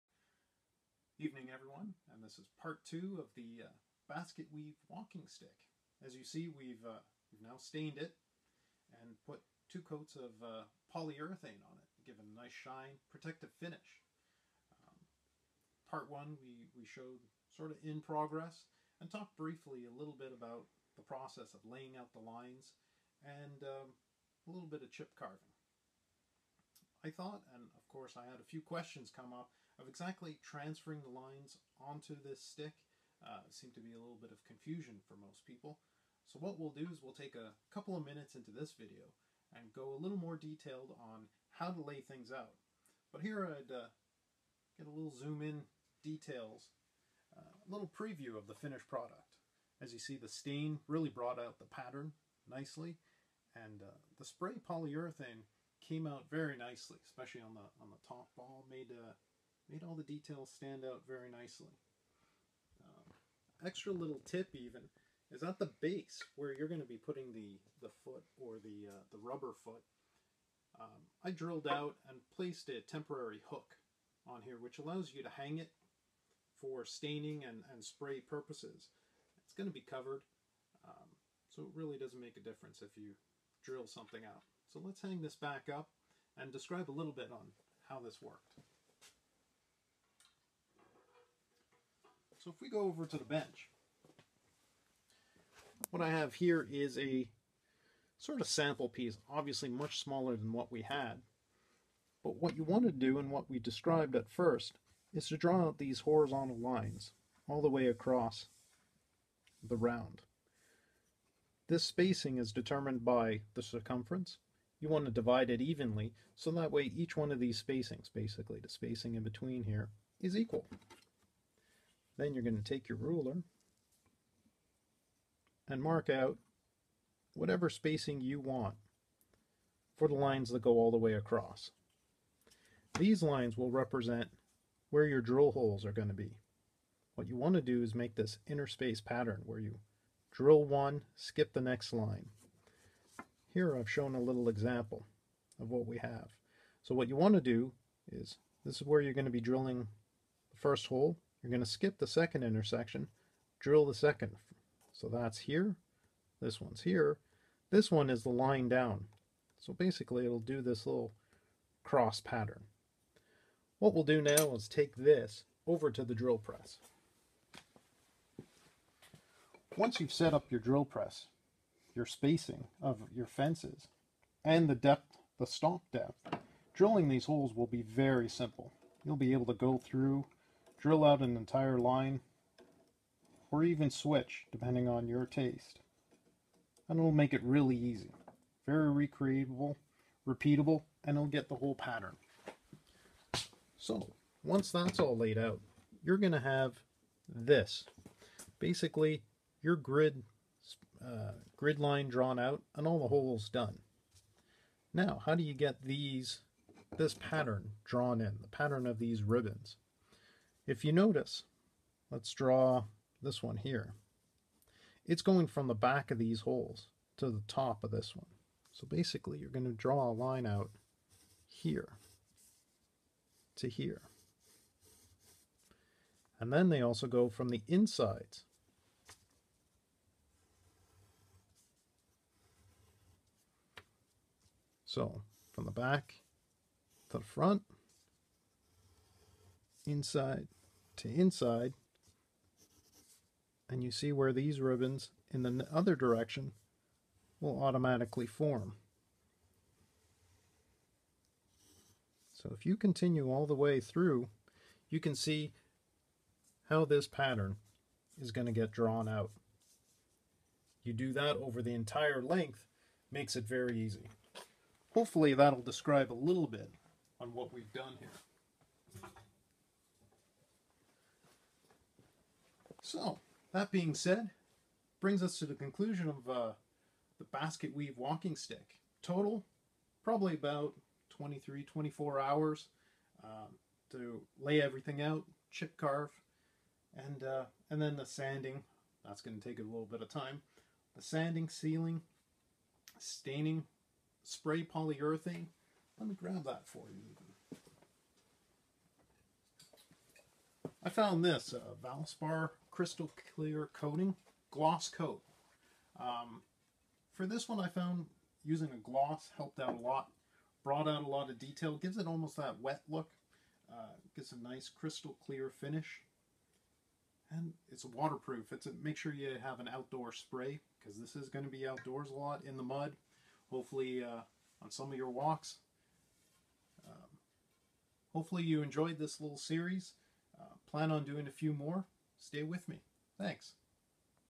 Basketweave Walking Stick Part 2
Part 2 of the Basket weave Walking Stick, we will go over the finished product and get into more details about laying out the pattern and transferring it to the round shape. Decided to keep it simple and not embellish the handle or base. Hopefully you enjoyed this project and let me know your thoughts and ideas for future projects. Thanks.
Also, please take time and see my second walking stick, the "Dragon Walking Stick" just released;
Thanks again!